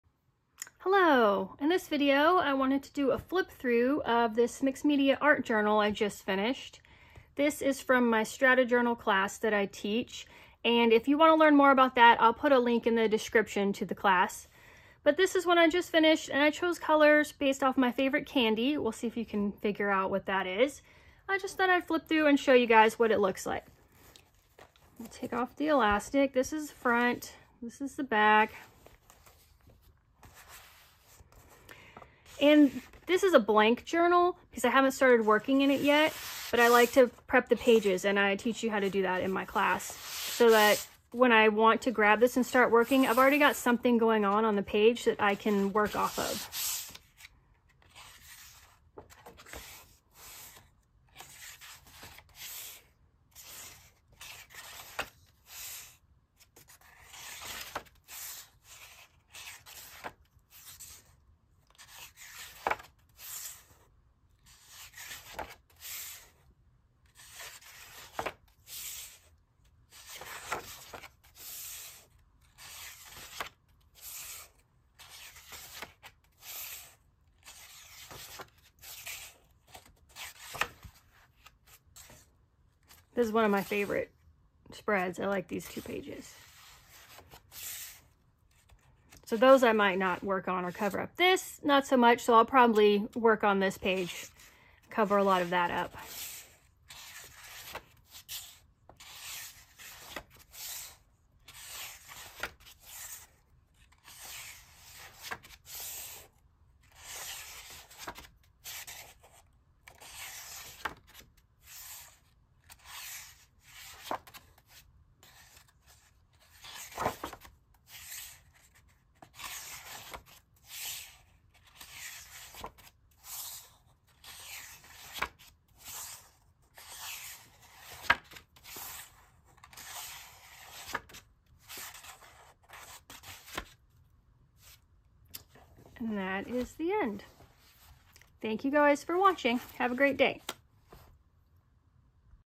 Strata Journal - Mixed Media Art Journal flip through
This is a video flip through of my “Reese’s” Strata Journal.  This journal is not for sale, but I am working on a new batch of journals that will be available for purchase soon.